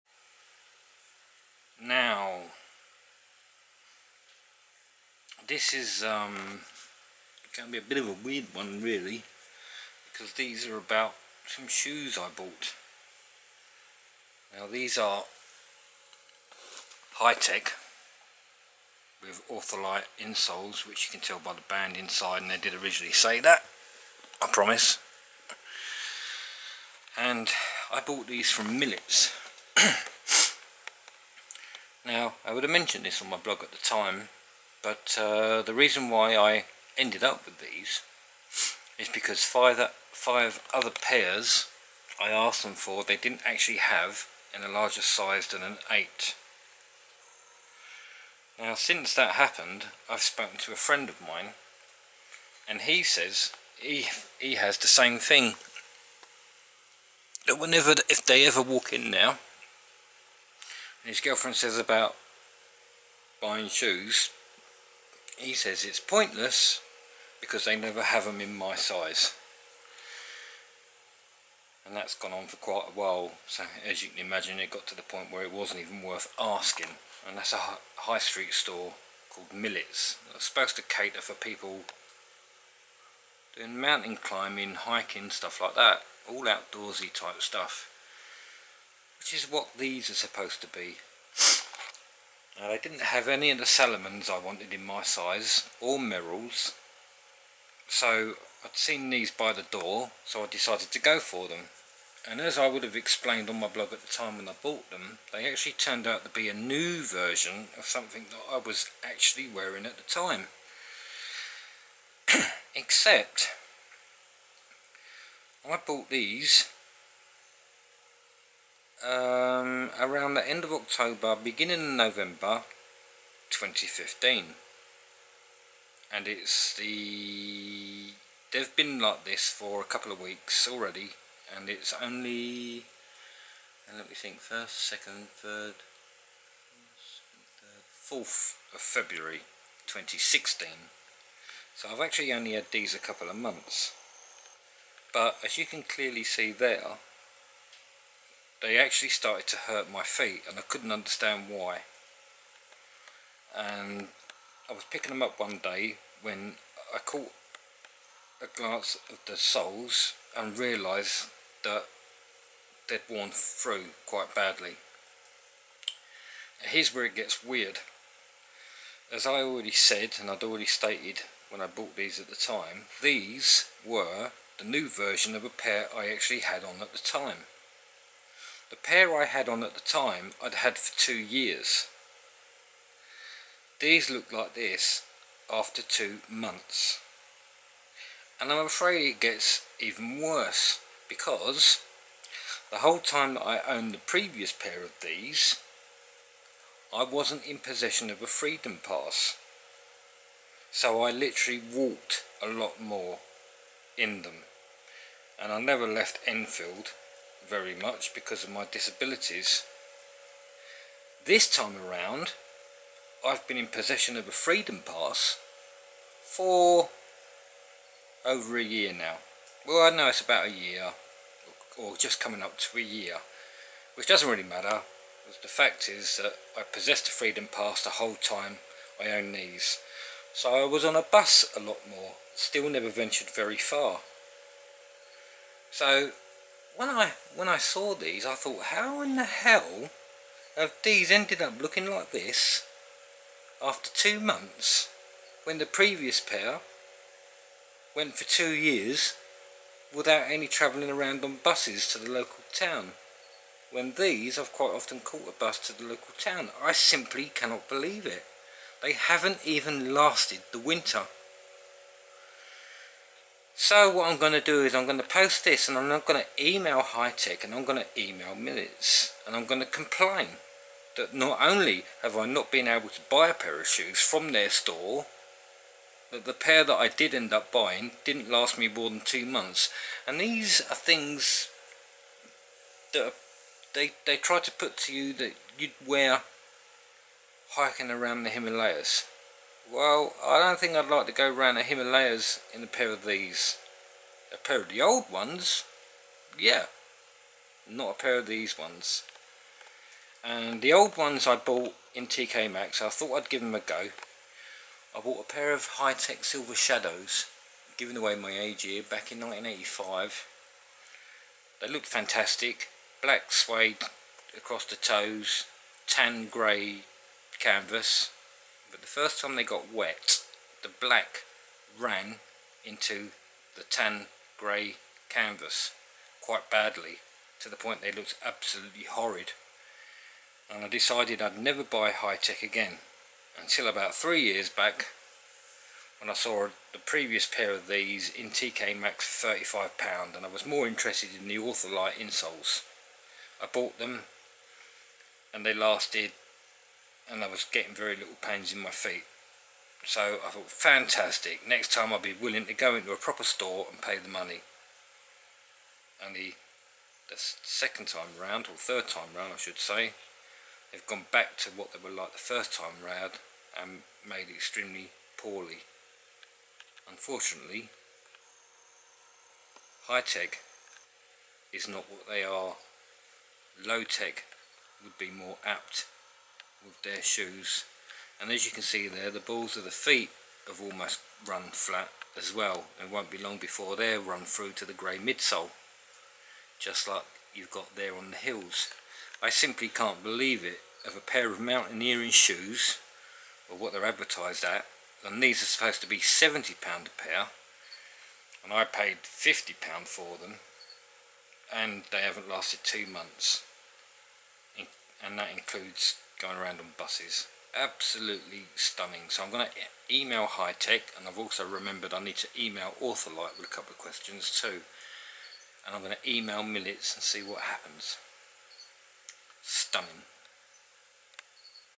Low-Tec Shoes
Bloody annoyed here!

These are a little over two months old despite the very same earlier design lasting two years, somehow?!

Worse still is that I have been in possession of a Freedom Pass, free bus and trains, for nearly a year while I did not with the -previous pair of shoes?!

What? How? I do not bloody know!

I will email both Hi-Tec and Millets and complain, as they previous five pairs of shoes I asked for they did not have in my size!

A friend now tells me that whenever he and his girlfriend go in Millets she tells him to get shoes and he replies "I am not bothering, they NEVER have my size, EVER!"

When I bought these they were the only type they had bigger than a size 8!

Idiots.

Was not just that store though as I asked them to order the missing pairs and they had none of them bigger than a size 8 anywhere in the UK?!

WHAAAAAAT?!